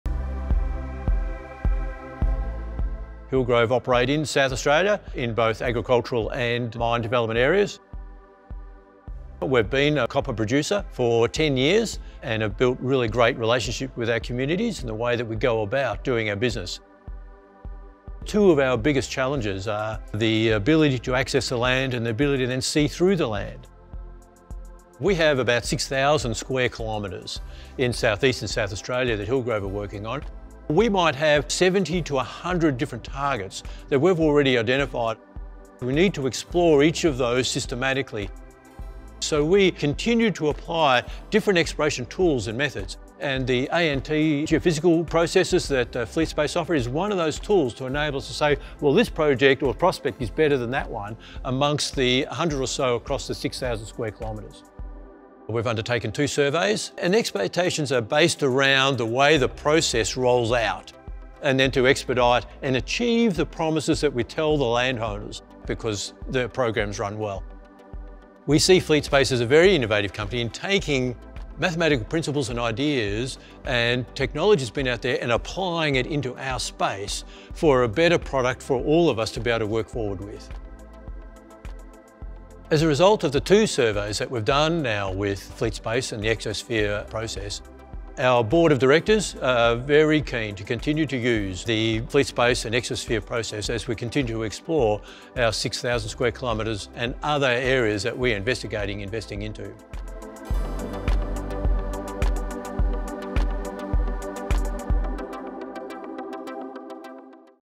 Hillgrove Resources Customer Champions
Client Success Series Part 2: Hillgrove Resources
ExoSphere is changing exploration!
A series of videos not from our own perspective but told in our clients' own words. Since we launched ExoSphere it has been a great journey to changeexploration, delivering results quickly and in a more sustainable way. It is even more rewarding when we get to hear that from our clients.

A big thank you to Hillgrove Resources for your valuable feedback.